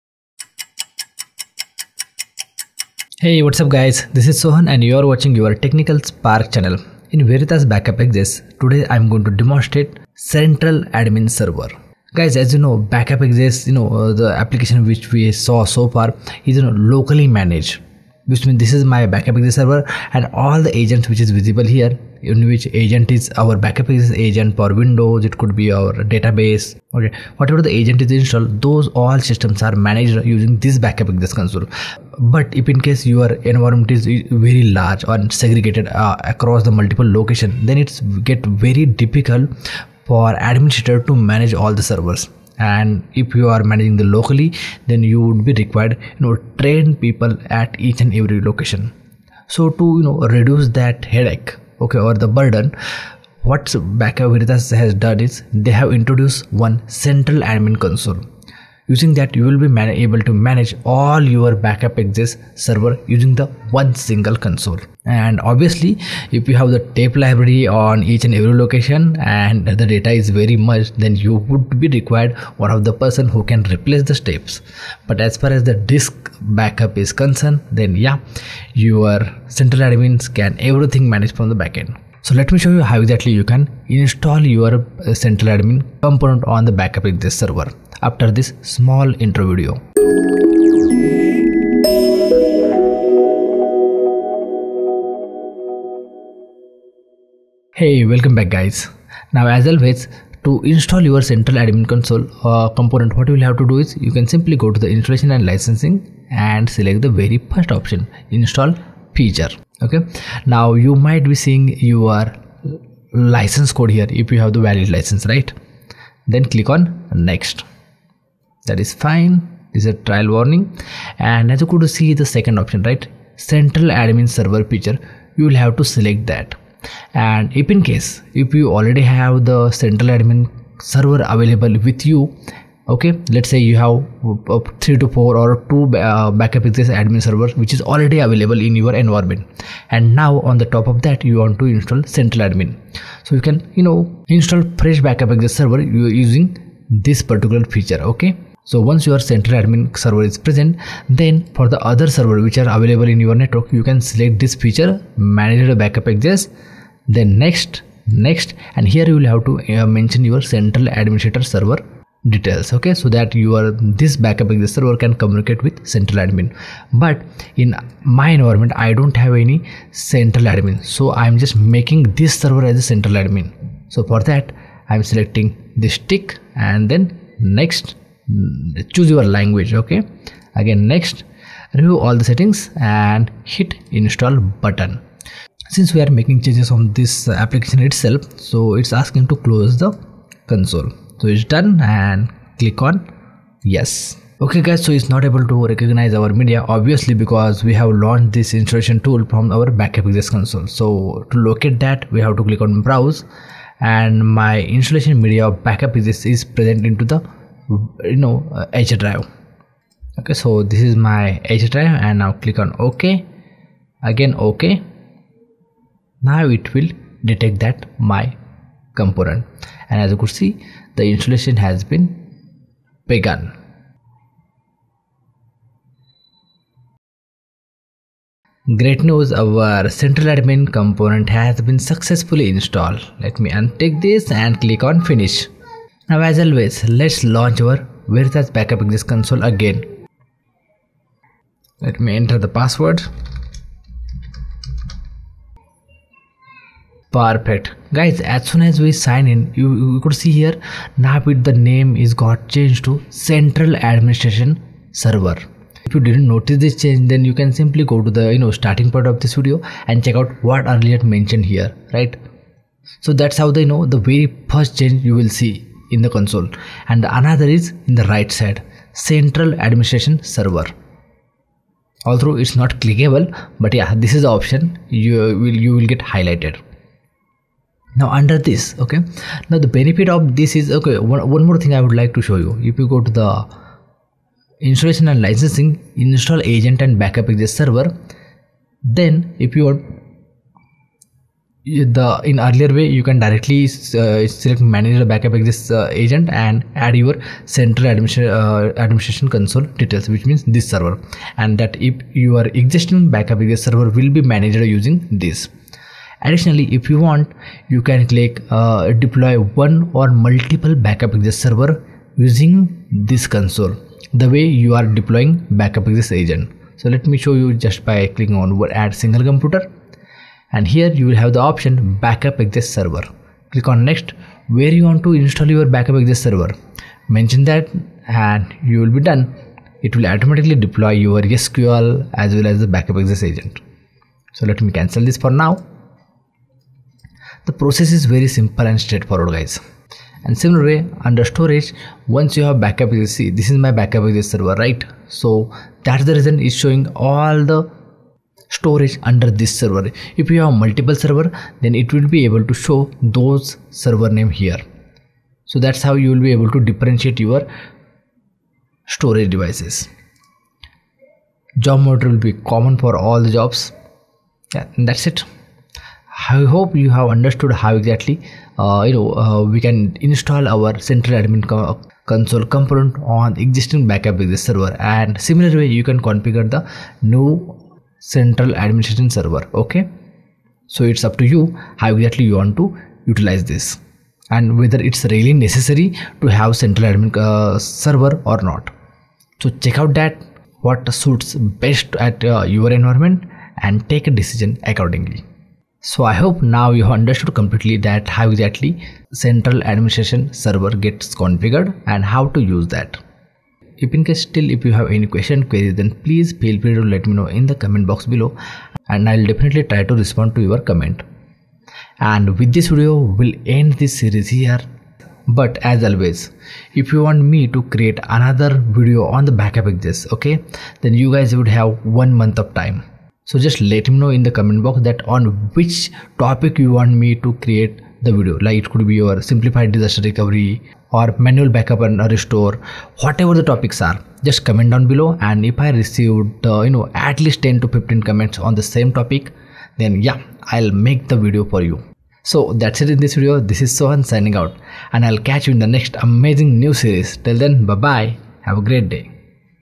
Unveiling Veritas Backup Exec Central Administration Server: Streamlining Data Management
🔑 Centralize your data management prowess! Join us in this comprehensive exploration of Veritas Backup Exec Central Administration Server, where we uncover the pivotal role it plays in streamlining backup operations. Whether you're an IT professional or a data administrator, this guide empowers you to harness the full potential of centralized administration for enhanced efficiency and control.

🛠️ Key Highlights:
 • Understanding the Concept and Benefits of Central Administration
 • Setting Up and Configuring Central Administration Server
 • Managing Multiple Backup Exec Servers from a Single Interface
 • Navigating Centralized Job Monitoring and Reporting
 • Leveraging Policy and Job Templates for Consistency
 • Pro Tips for Effective Centralized Backup Operations

Empower yourself with the knowledge needed to optimize your backup infrastructure with Veritas Backup Exec Central Administration Server. Streamline your data management processes and ensure the resilience of your organization's data with centralized administration! 💻💡

This video is uploaded for Education purpose only.